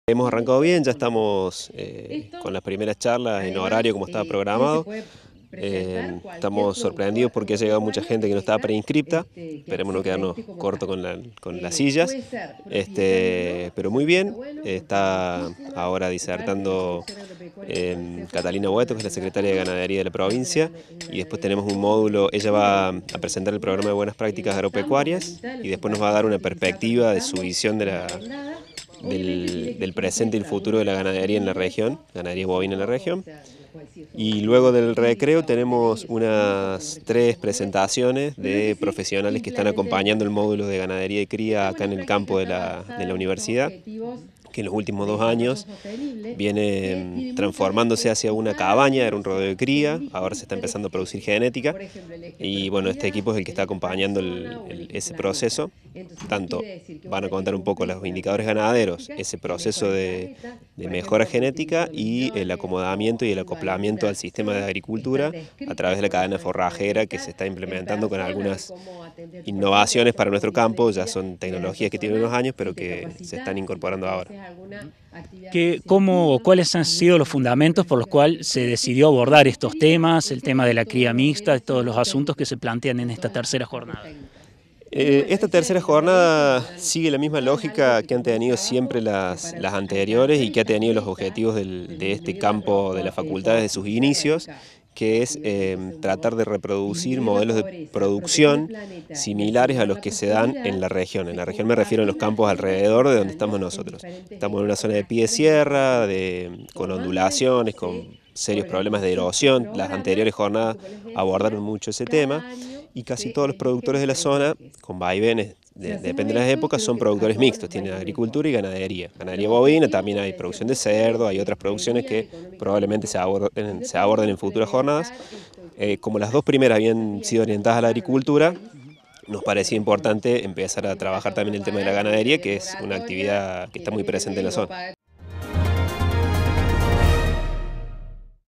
SE DESARROLLARON LAS III JORNADAS TÉCNICAS DE LA FACULTAD EN EL CAMPO POZO DEL CARRIL
MAURO MACIÓ – SECRETARIO DE EXTENSIÓN FAV